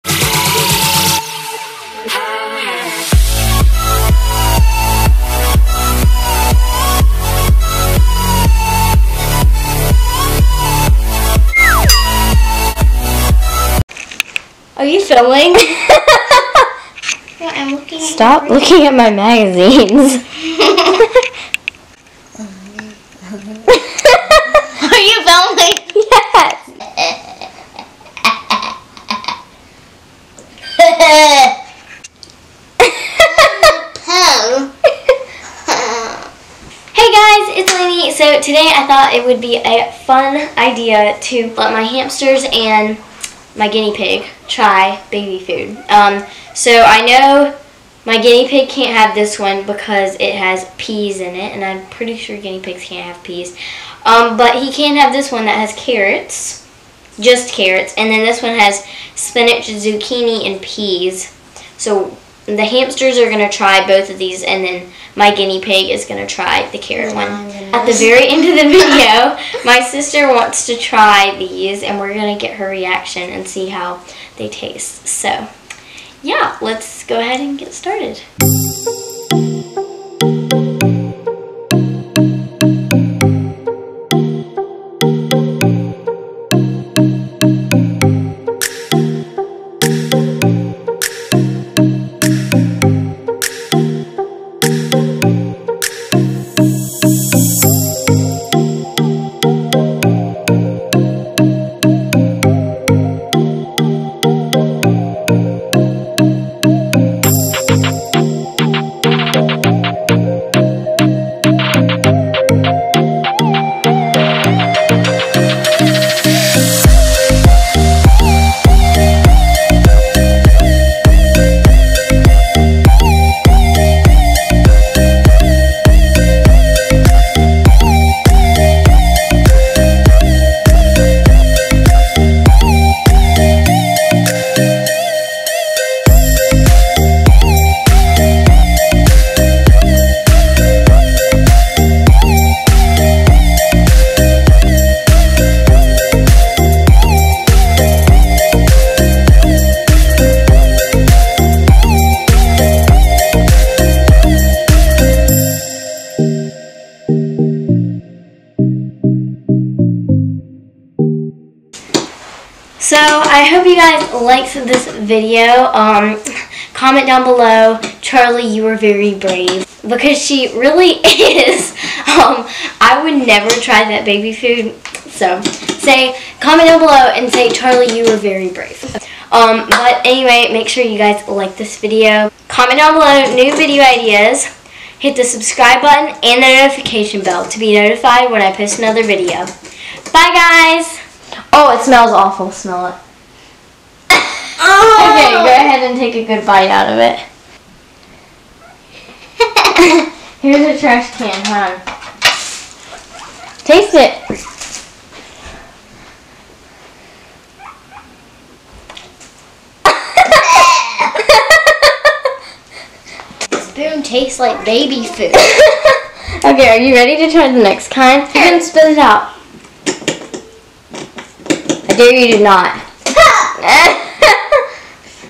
My Pets Try Baby Food | Fluffy Hammy
hope everyone enjoyed the video. This was so much fun to film. Stay tune till the end when Charlee tries the baby food.